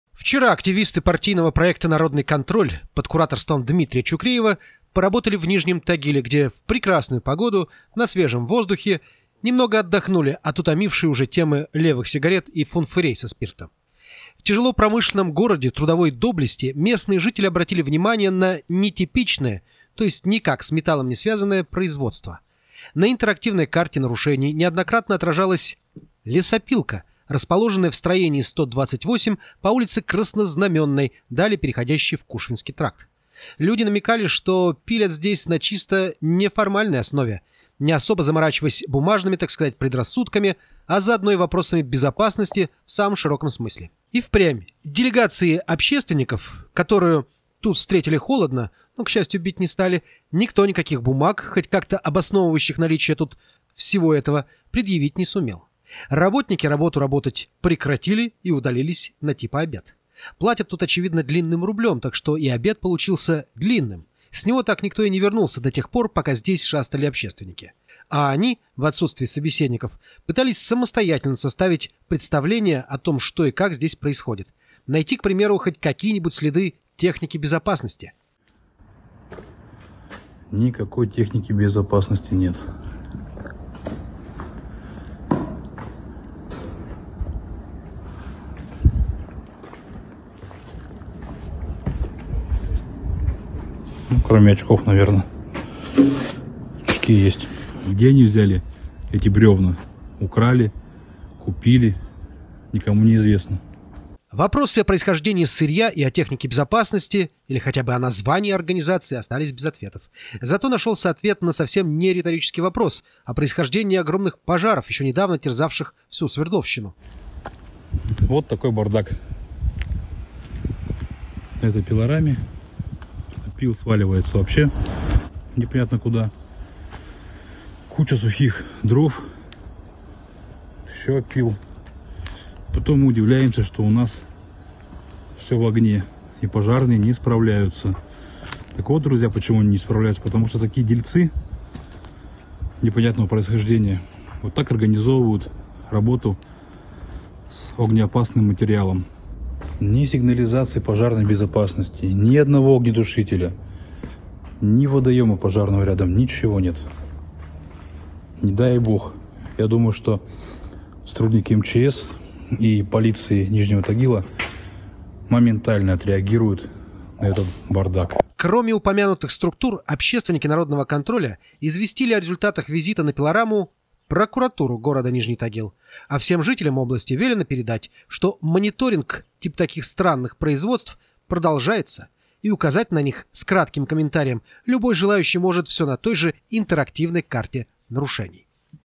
Лесопилка как источник мегапожара.
"Народный контроль" нашёл такую на окраине Нижнего Тагила.